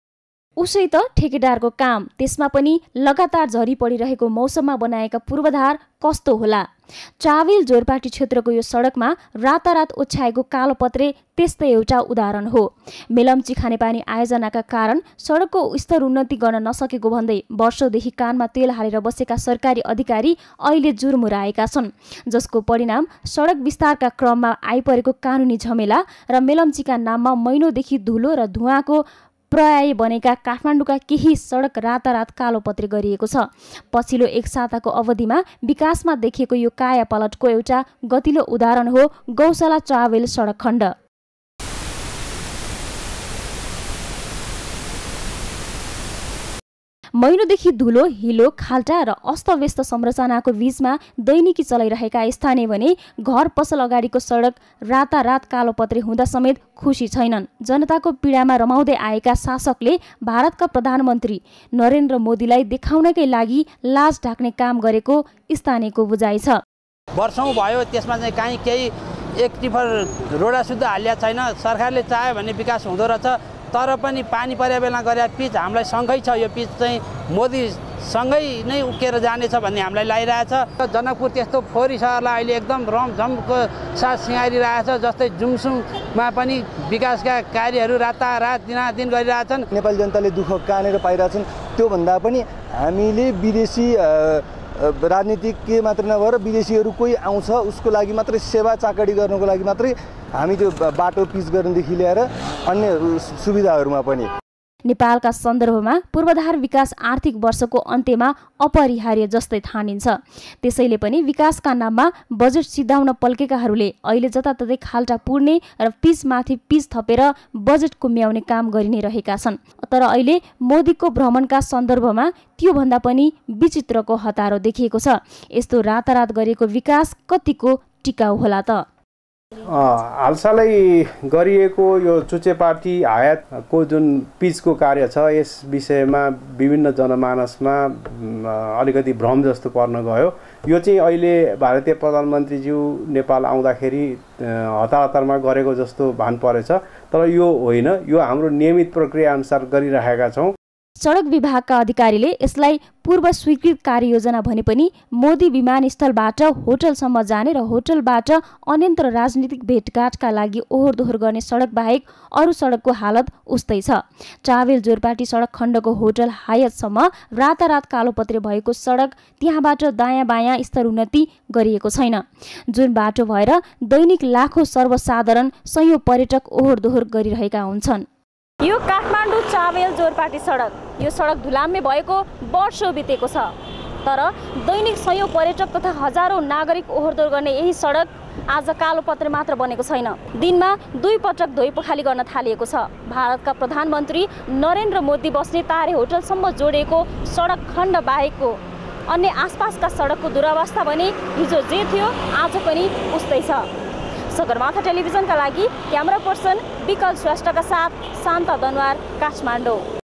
मोदी आउादा सडक मर्मत
नेपालीमा एउटा भनाई छ, फाटेको सुरुवाल टाल्न पनि सम्दि भेट नै जुर्नुपर्छ । यो नेपाली समाजको बिडम्वना हो । अहिले मुलुकका केहि भागमा सरकारि अधिकारिहरु यसैगरी दिनरात नभनि विकास निमार्णमा खटिएका छन् । आर्थिक बर्षको अन्त्यमा बजेट सिध्याउने चटारो संगै, भारतका प्रधानमन्त्री नरेन्द्र मोदी यही मौषममा नेपाल आउने भएपछि विकासका नाममा तअर काट्न पल्केकाहरुको व्यस्तता बढेको हो ।